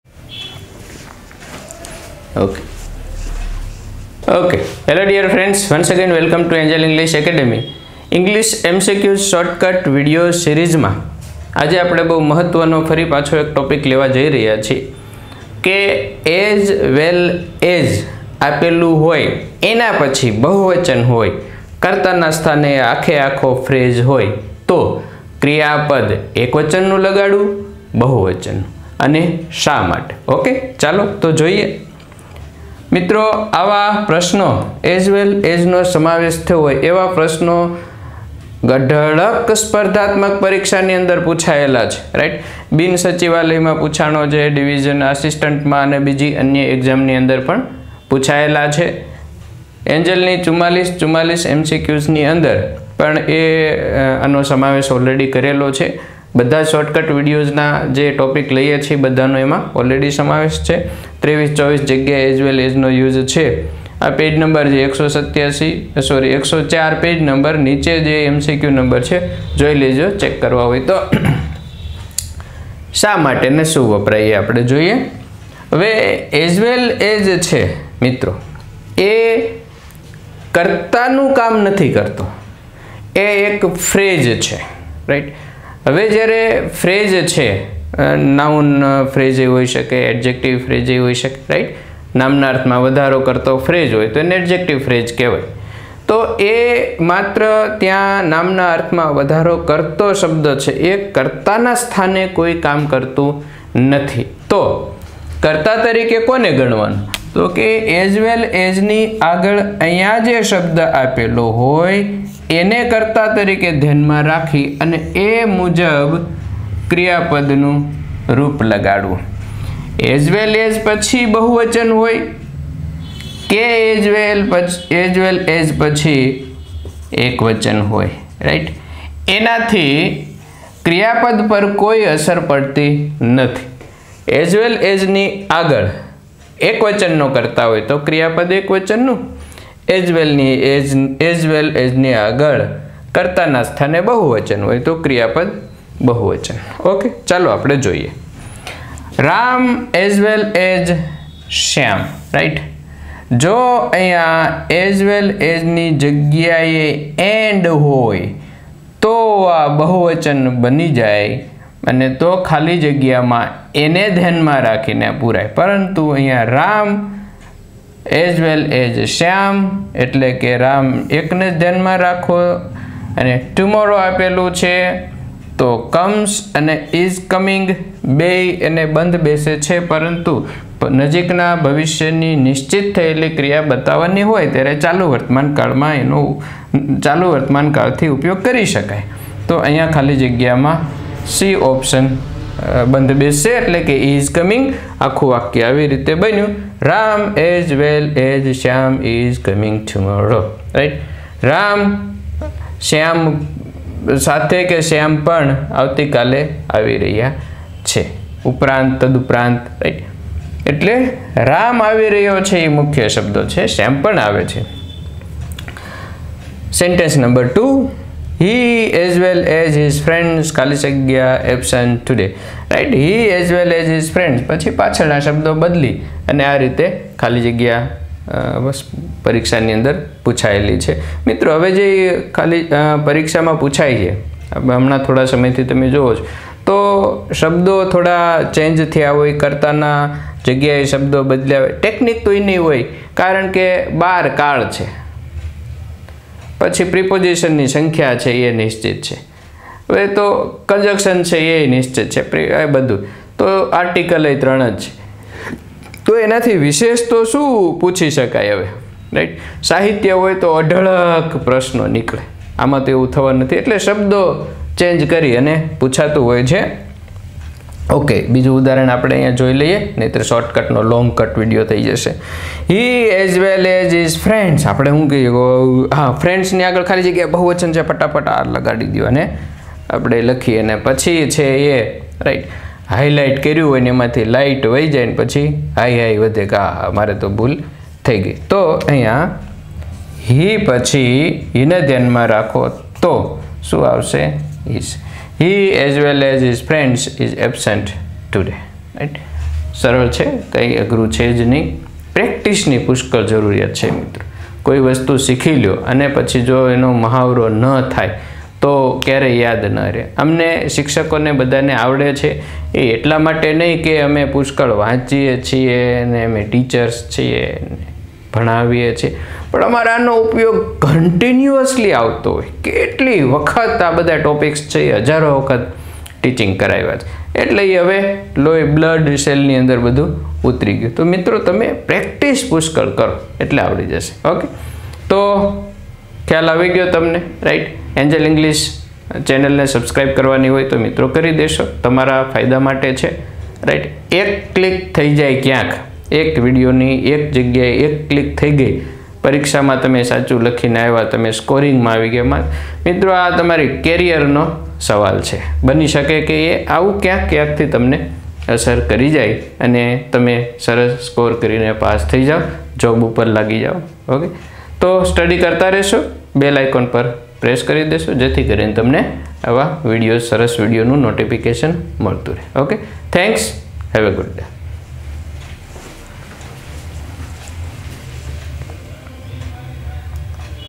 English MCQs Short Cut Video-22 'as well as' હોય ત્યારે કર્તા એકવચન ગણવો કે બહુવચન | Kishan Sir
English MCQs Short Cut Video-22 'as well as' હોય ત્યારે કર્તા એકવચન ગણવો કે બહુવચન

Angel’s Online English Services